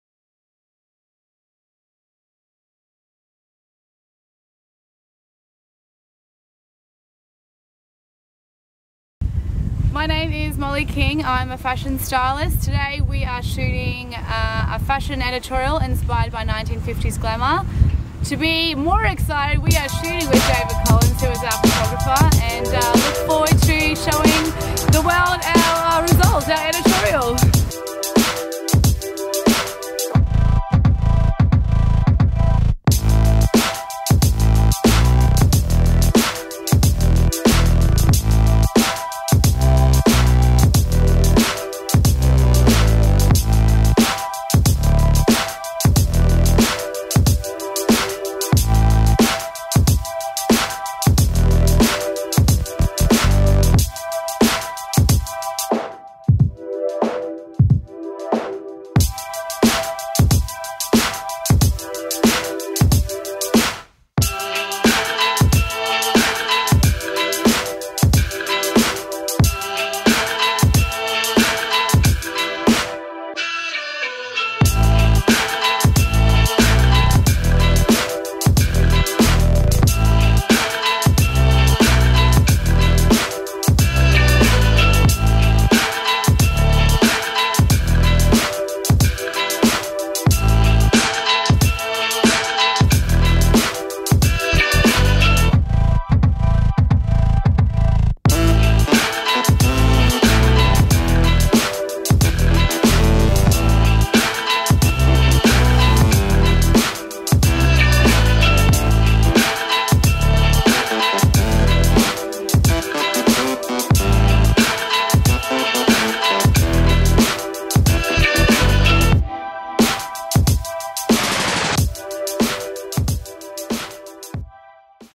David Collins Photography (Fashion Shoot Part 2) by Hawke Anderson
David Collins Photography and Stylist Molly King team up with Model Sammi Wallis for this 1950's inspired photo-shoot. Hair and Make-up by Stephan. Music by Exploding Koala. Shot and Produced by Hawke Anderson.